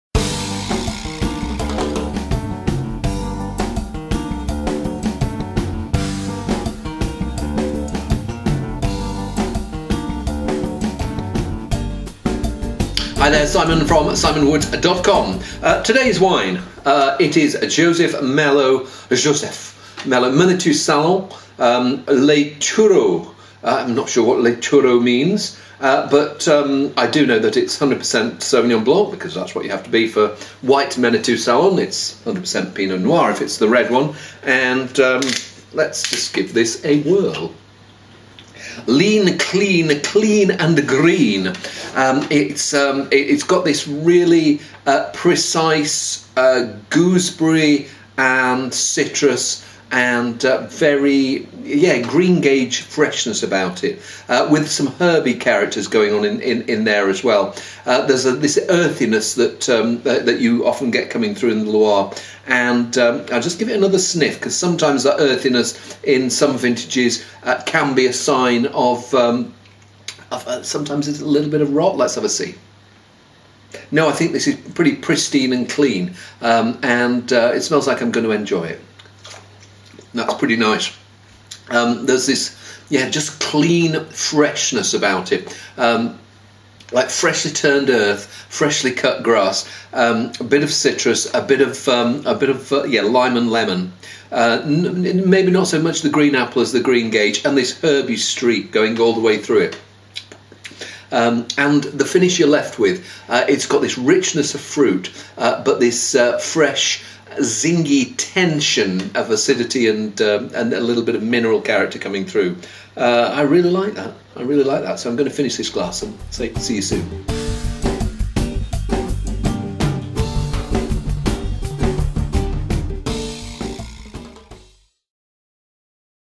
Wine Tasting with Simon Woods: Joseph Mellot Menetou Salon Les Thureaux 2013, Loire, France
Simon Woods finds a zingy Loire Sauvignon to stimulate his gastric juices...